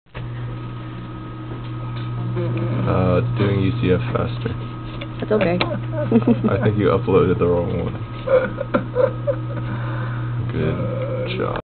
Embedded Systems Lab 7 -- UCF
7 segment LED, 7 resisters, 8 channel buffer and a MC6811 embedded processor